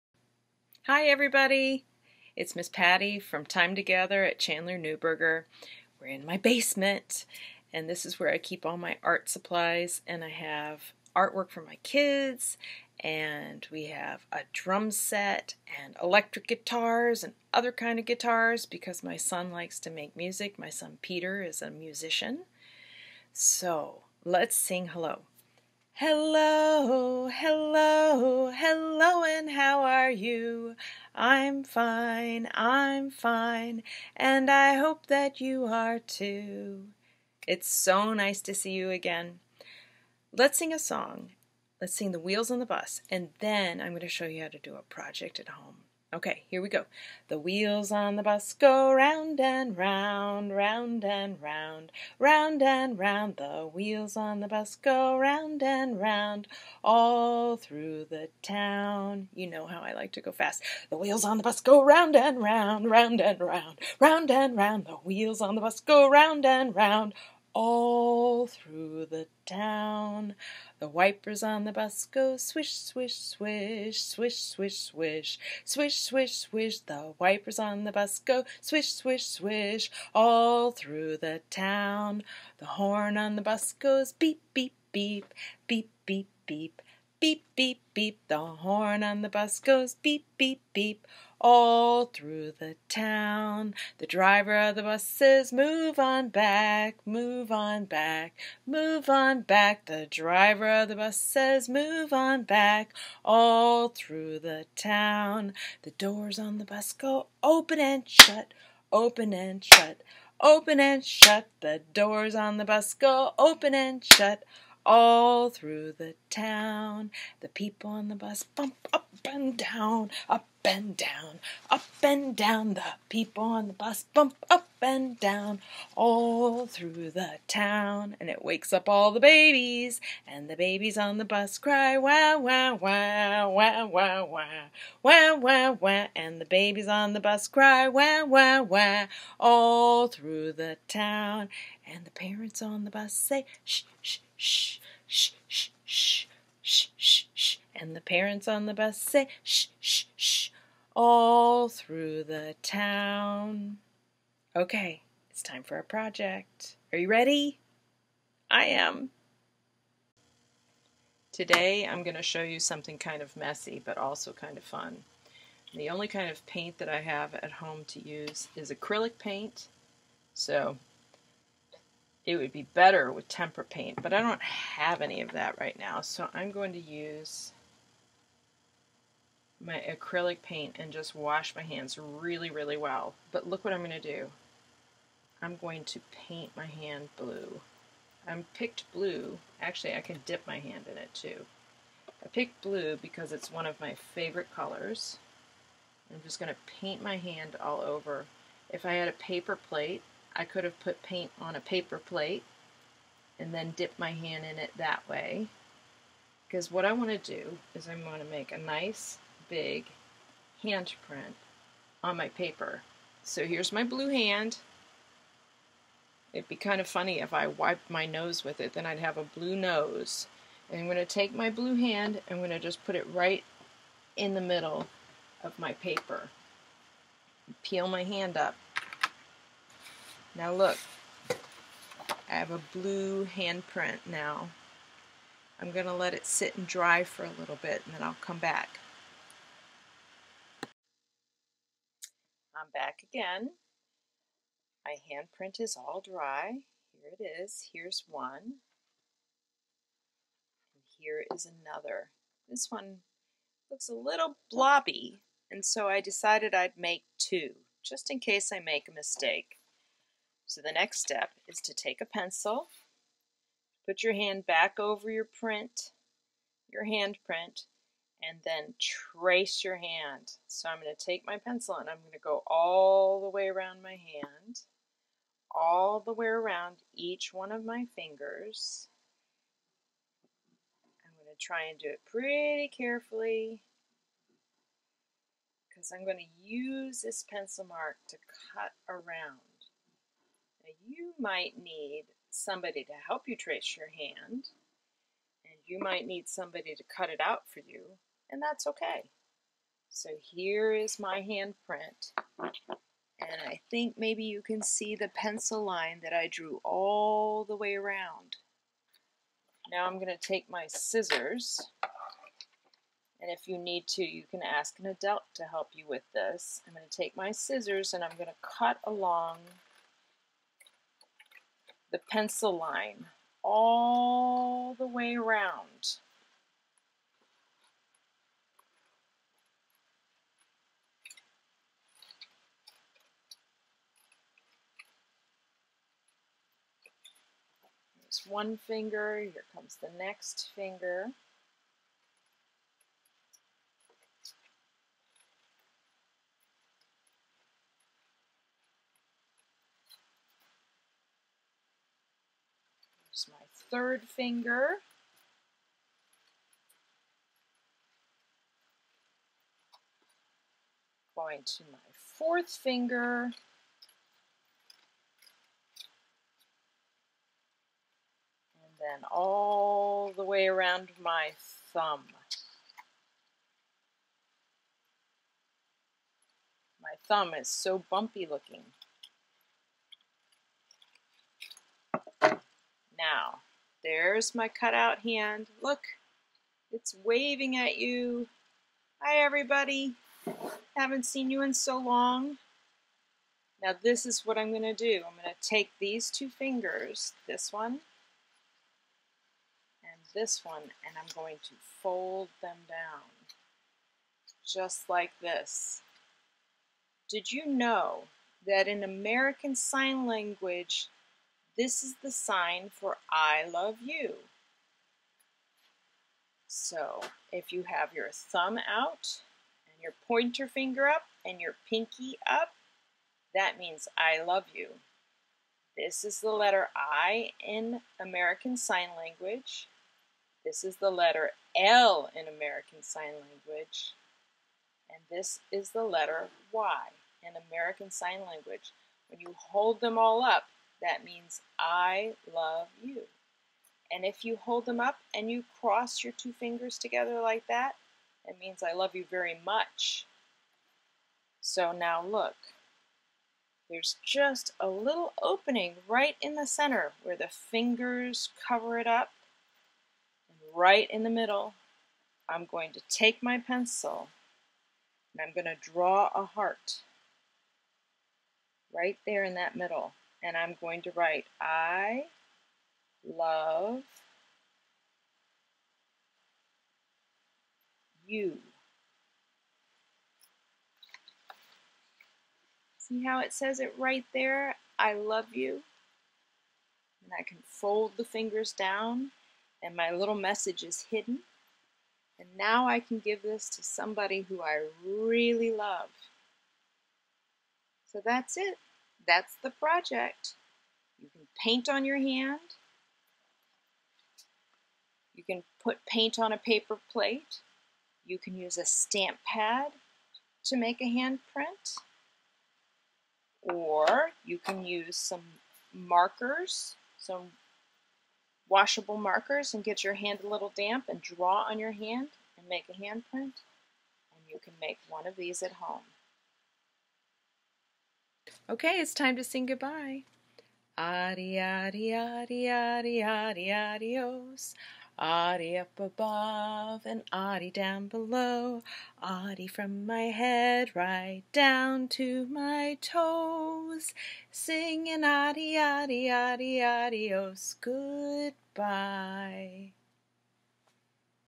I Love You Project with Miss Patti from Chandler-Newberger
Hi everyone! In this video, I present a fun, easy project to do with your child. It requires paper, a pencil, scissors and paint or an ink stamp pad. I did this years ago with my son when he was very small as a present for my husband.